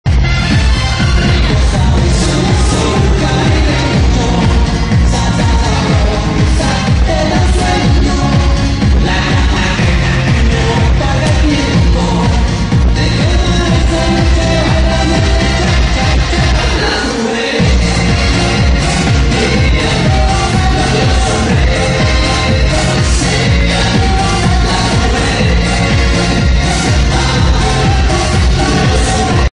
2011 Open Professional Rising Star Latin - Oleksandr and Oksana
U.S. Open Professional Rising Star International Latin Competition (C/S/R/PD/J)

Oleksandr Altukhov [448] with Oksana Dmytrenko